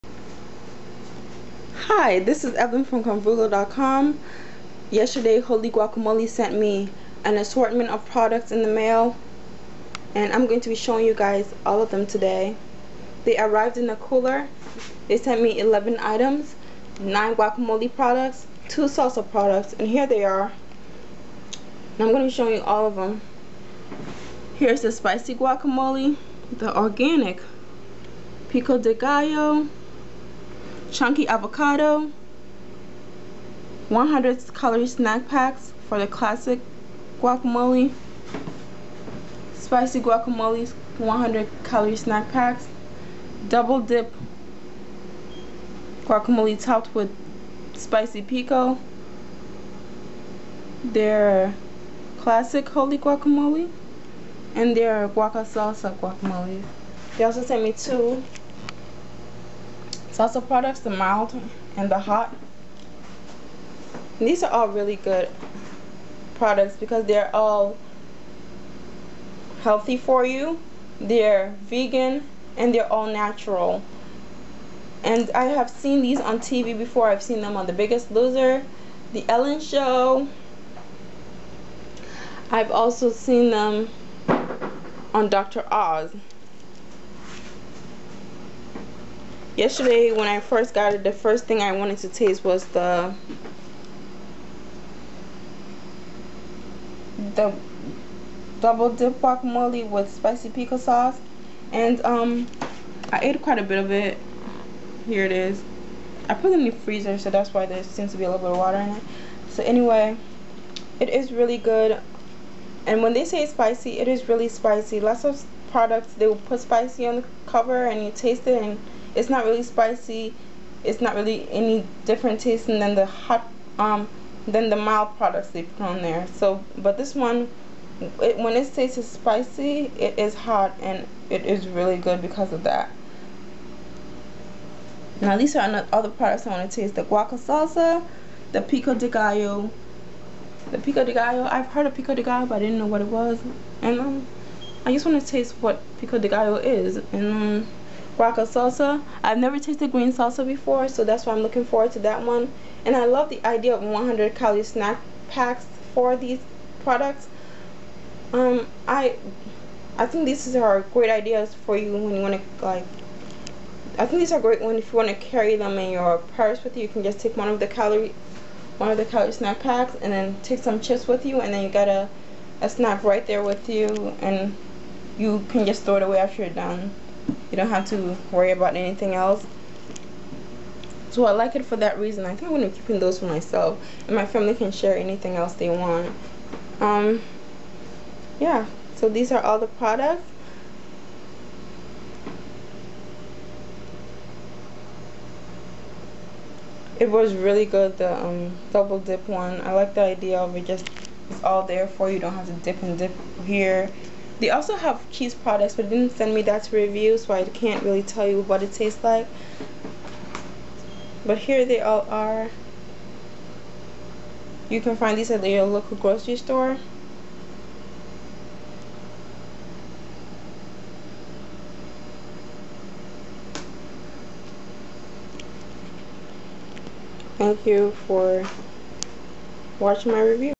Wholly Guacamole Review
My review for Wholly Guacamole. I was sent 11 products to review and give my opinion on and do a giveaway on my blog. The giveaway will happen next week but I wanted to show my readers the products I got.